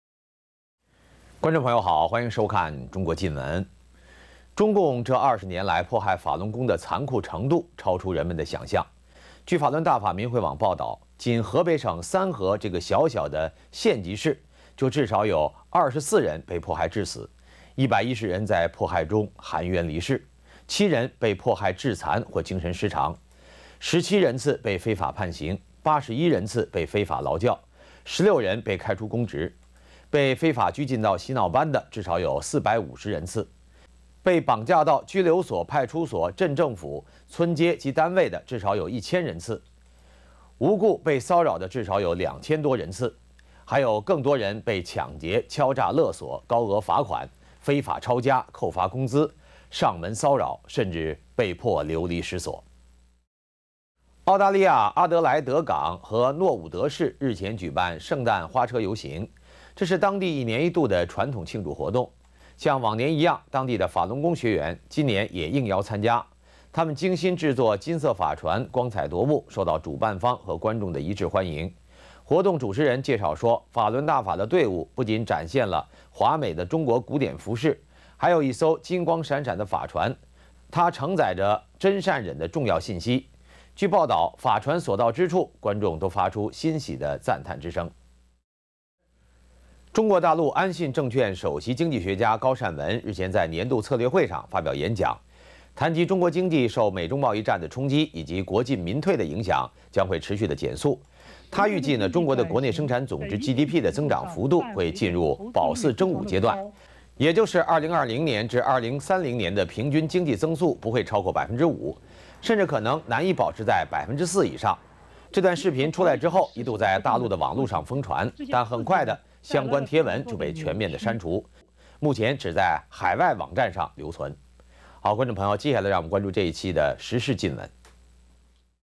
河北三河市24名法轮功学员被迫害致死【制止迫害法轮功】
【新唐人北京时间2019年12月06日讯】河北三河市24名法轮功学员被迫害致死

中共这20年来迫害法轮功的残酷程度超出人们的想象。据「法轮大法明慧网」报导，仅河北三河这个小小的县级市，就至少有24人被迫害致死，110人在迫害中含冤离世，7人被迫害致残或精神失常，17人（次）被非法判刑；81人（次）被非法劳教；16人被开除公职；被非法拘禁到洗脑班的至少有450人次；被绑架到拘留所、派出所、镇政府、村街及单位的至少有1000人次；无故被骚扰的至少2000多人次。还有更多人被抢劫、敲诈勒索、高额罚款、非法抄家、扣发工资、上门骚扰、甚至被迫流离失所。

南澳圣诞游行 法轮功金色法船受欢迎

澳大利亚阿德莱德港（Port Adelaide）和诺伍德市（Norwood）日前举办圣诞花车游行，这是当地一年一度的传统庆祝活动。

像往年一样，当地法轮功学员今年也应邀参加，他们精心制作金色法船，光彩夺目, 受到主办方和观众的一致欢迎。

活动主持人介绍说：「法轮大法的队伍不仅展现了华美的中国古典服饰，还有一艘金光闪闪的法船，她承载着『真善忍』的重要信息。」

据报导，法船所到之处，观众都发出欣喜的赞叹。

预测经济减速 大陆经济学家言论被封

大陆安信证券首席经济学家高善文，日前在年度策略会上发表演讲，谈及中国经济受美中贸易战冲击、以及「国进民退」的影响，将会持续减速。他预计, 中国的国内生产总值GDP增长幅度，会进入「保四争五」阶段，也就是2020至2030年平均经济增速不会超过5%，甚至可能难以保持在4%以上。

这段视频出来之后，一度在大陆网络上疯传，但很快, 相关贴文就被全面删除，目前只在海外网站上留存。

编辑/周玉林